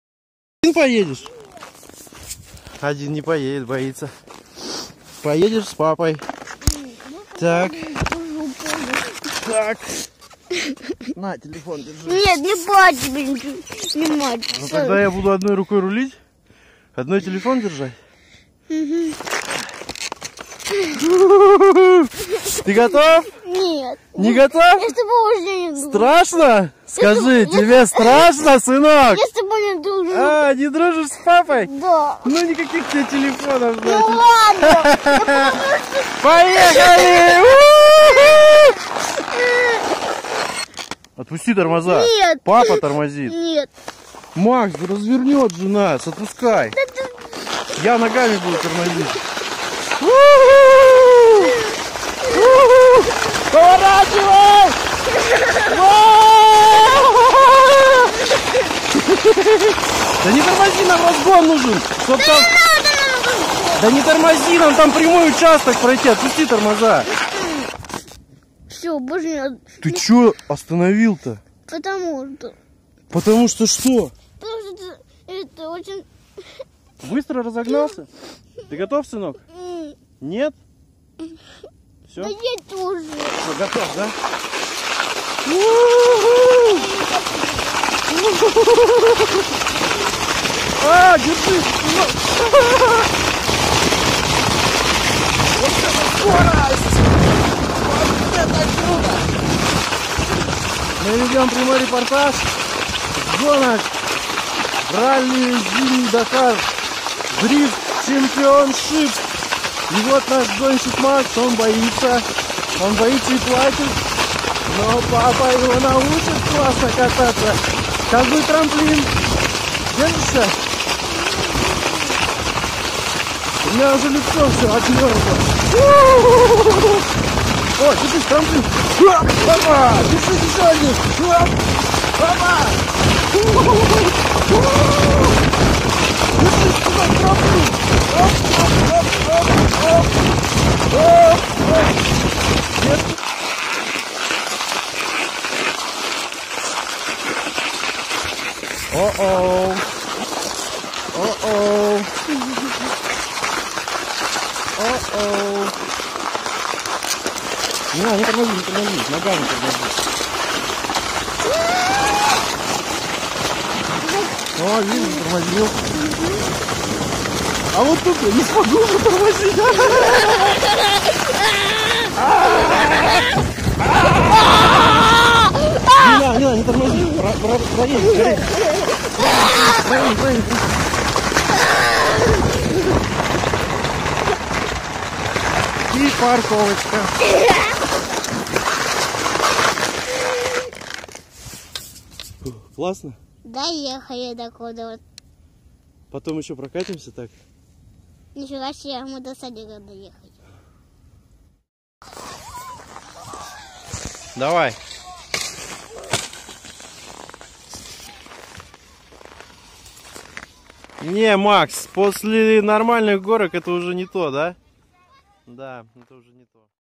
Зимние покатушки на аргамаке
Спуск по дороге от метео станции